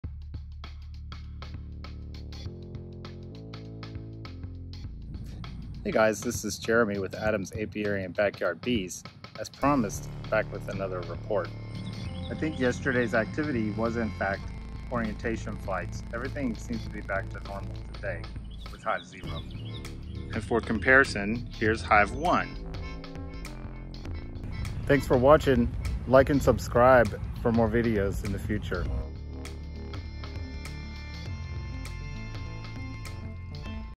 Hive Status Update
The other day a bunch of bees were flying around Hive0's entrance. I suspected some new field bees were out doing orientation. I hold by that though today.

(CC) Adams' Apiary & Backyard Bees